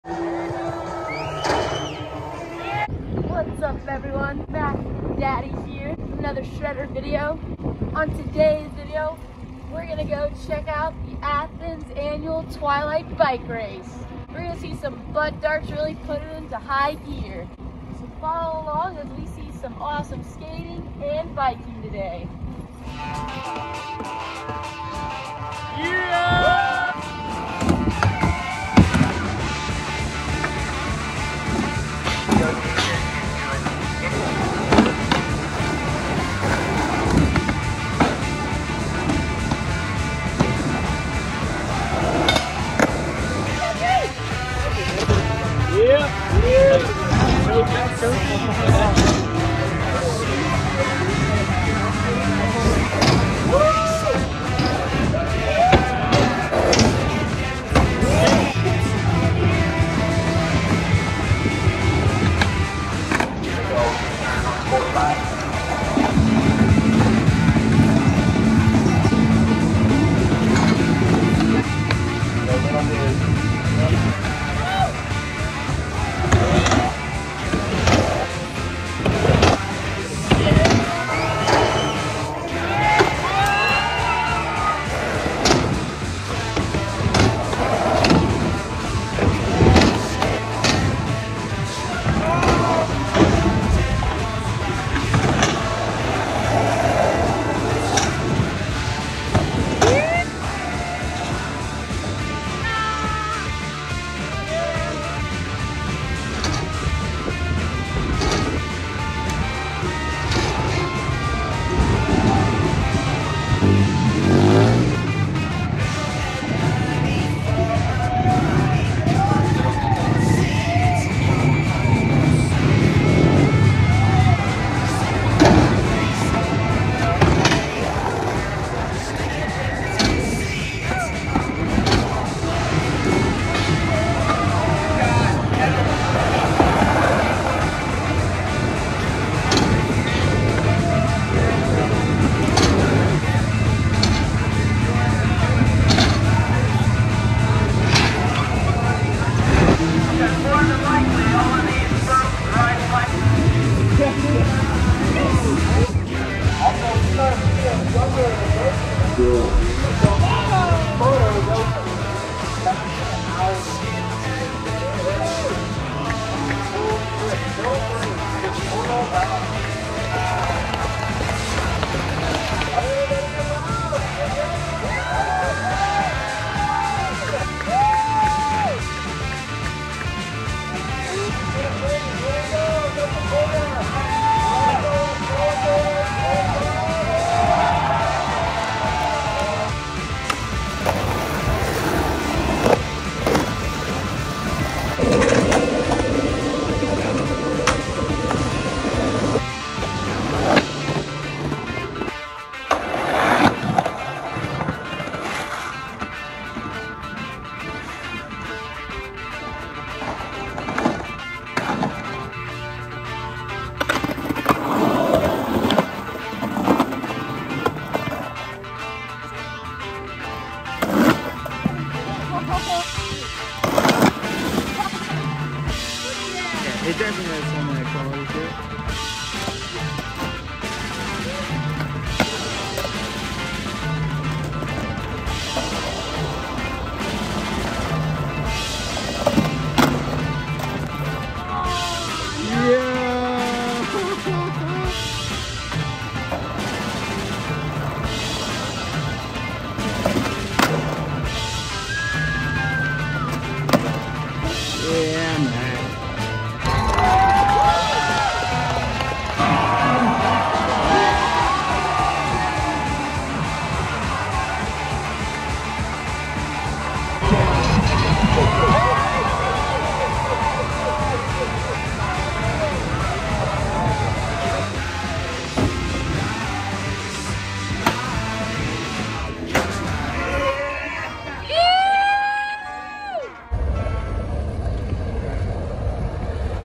Is this the CRAZIEST skate event EVER?!
Watch these local rippers tear it up during Athens Annual Twilight Bike Race.

You'll see some amazing skating and biking!!